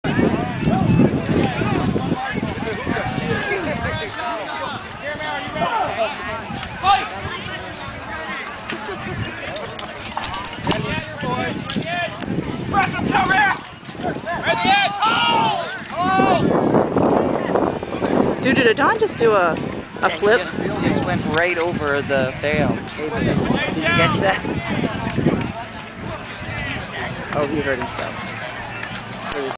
5 man winners '08; round 03
These are clips of the winning team of the Atlantian 5-Man Melee Tournament, representing the Barony of Caer Mear, Atlantia: Dante di Pietro, Aedan Aylwyn, Dominyk Wolferam, Celric d'Ravelle, and Armand of Caer Mear.

Videos courtesy of Dame Rosalind Delamare.

Note the end where Aedan goes airborn and lands on me.